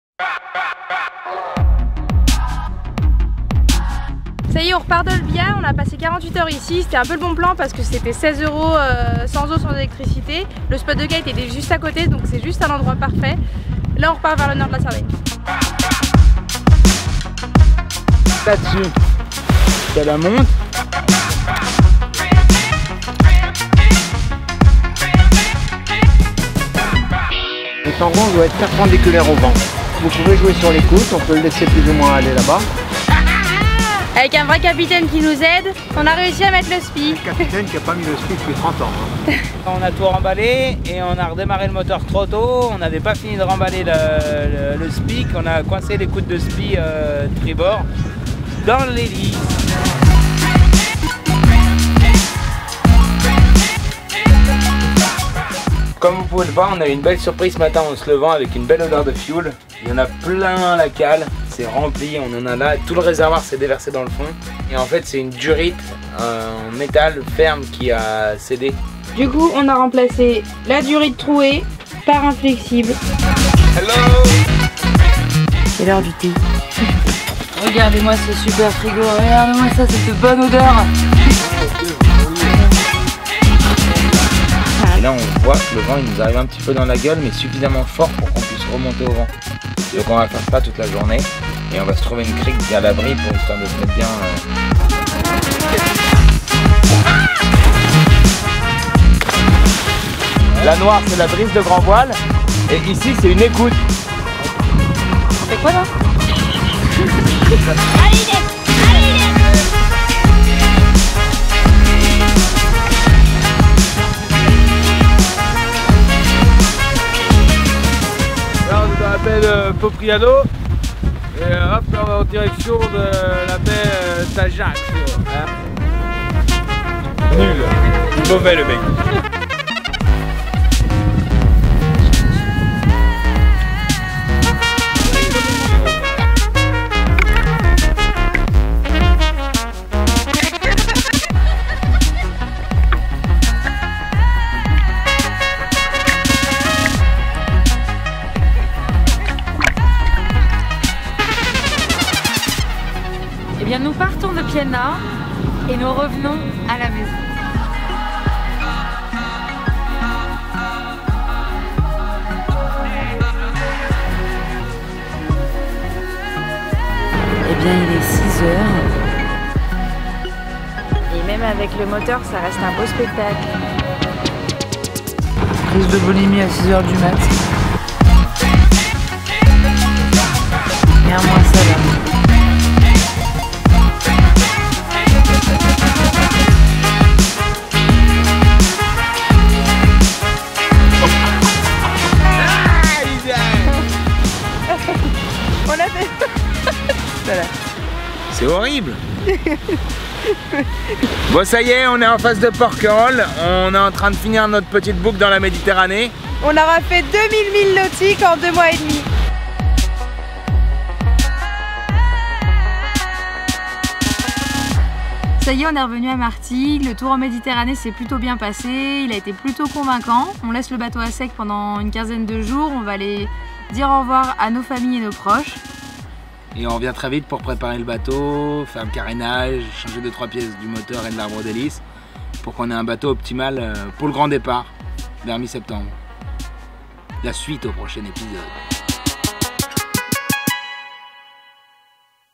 270 OUEST #08 : Retour de Mediterranée
Fin de l'entrainement, nous rentrons de Méditerranée pour préparer le projet.